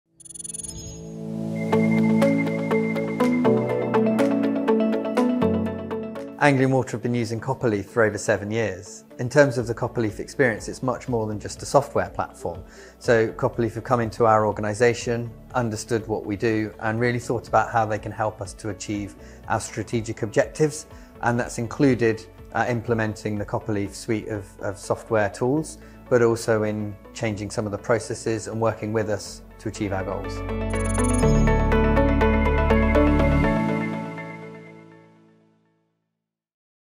Speaker Announcement - John Green, Anglian Water | AIPM Forum - London
We are delighted to announce John Green, Strategic Value Manager at Anglian Water, will be speaking at our Asset Investment Planning and Management (AIPM) Forum in London on 5th December.

Click here to find out more and register for the event